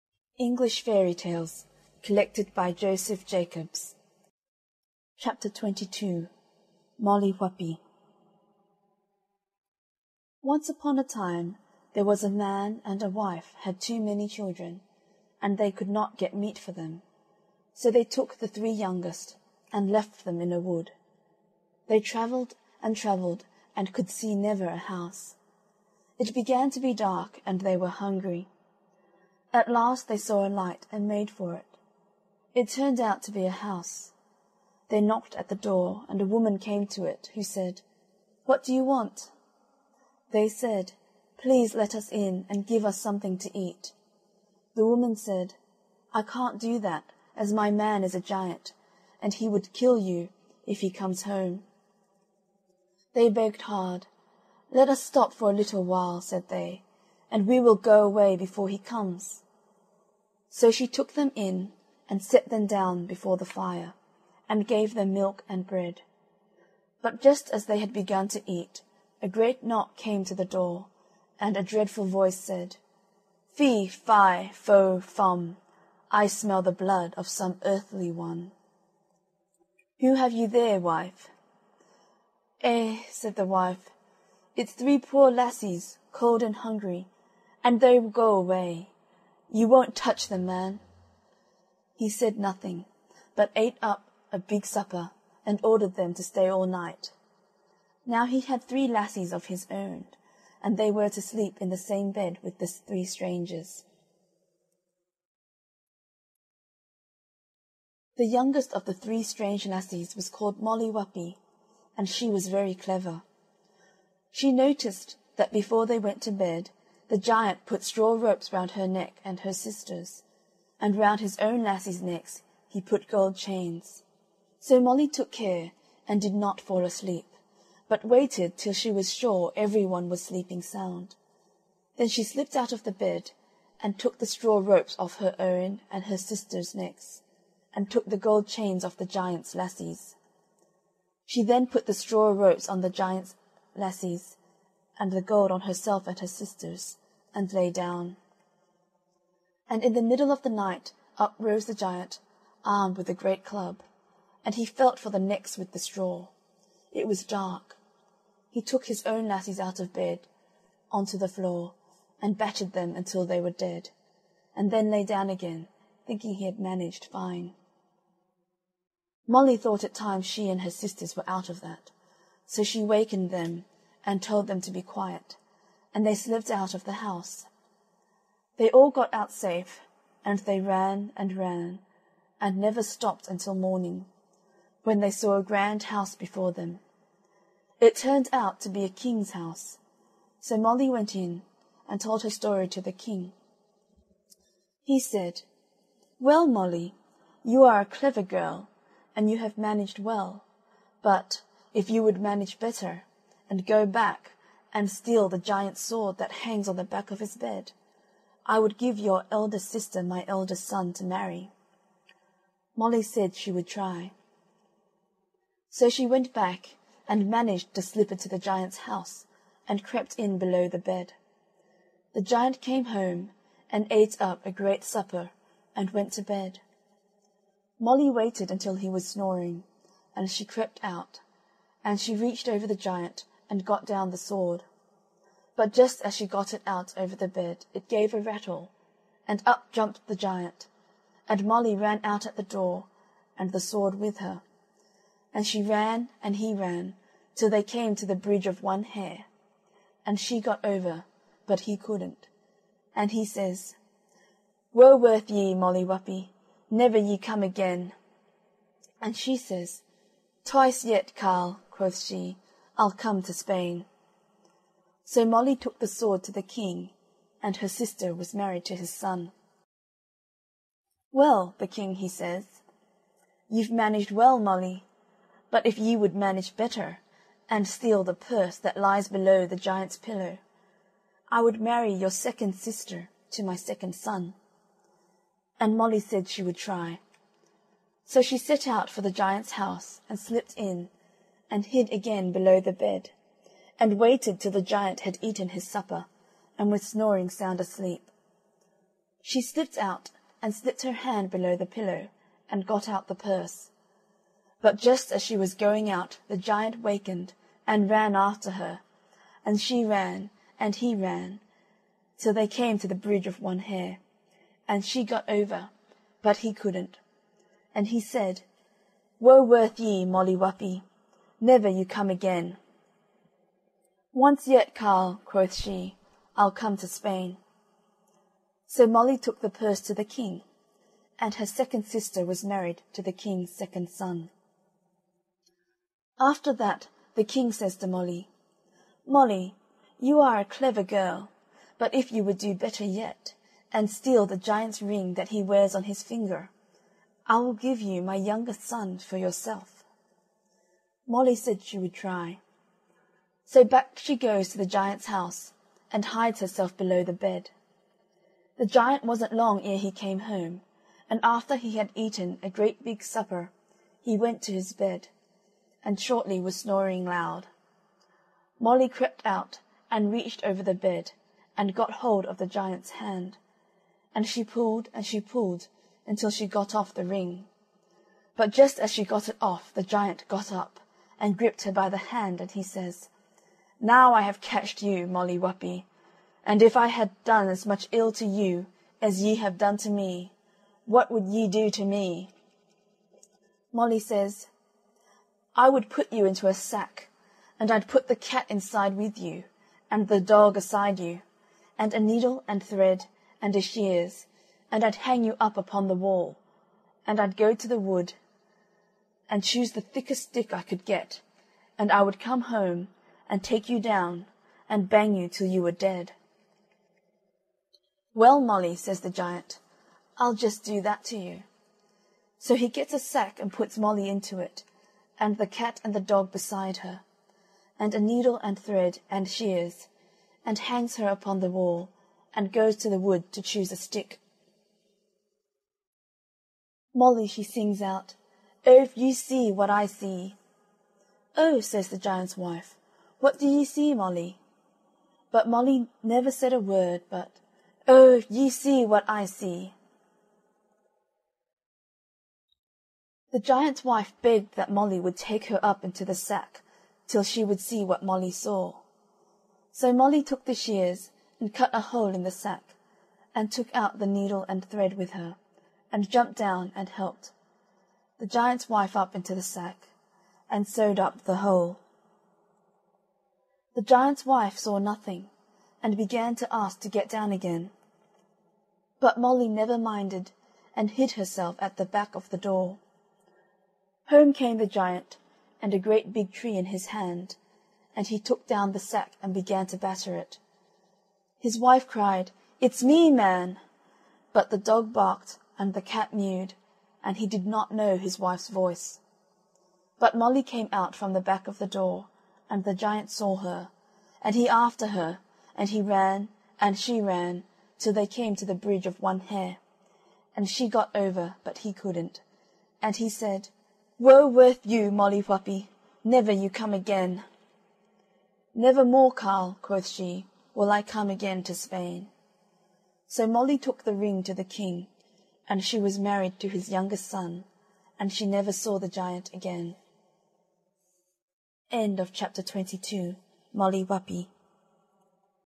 Chapter 22 - English Fairy Tales by Joseph Jacobs
Chapter 22: Molly Whuppie. Classic Literature VideoBook with synchronized text, interactive transcript, and closed captions in multiple languages. Audio courtesy of Librivox. Read by Joy Chan.